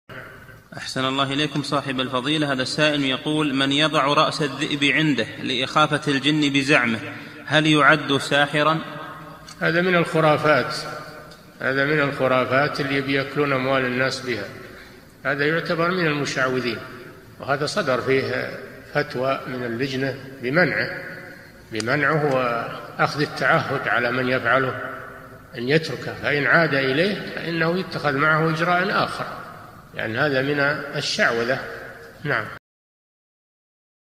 [249 -954] مَن يضعُ رأس الذئب عنده لإخافة الجن بزعمِهِ، هل يُعد ساحرًا؟ - الشيخ صالح الفوزان
-[249 /954] السؤال: مَن يضعُ رأس الذئب عنده لإخافة الجن بزعمِهِ، هل يُعد ساحرًا؟
الجواب: هذا من الخرافات الذي يأكلون أموال النَّاس بِهَا، هذا يعتبر من المشعوذين، وهذا صدر فيه فتوى من اللجنة بمنعِهِ وأخذ التعهُّد على من يفعله أن يتركه، فإن عاد إليه فإنه يُتخذ معه إجراءٌ آخر، لأن هذا من الشعوذة.
للاشتراك في قناة الشيخ صالح الفوزان
سؤال وجواب من شرح منظومة الآداب ( 945 ) مقطع

للاستفسار والإقتراحات والدعم
رئيس الفريق
لمشروع كبار العلماء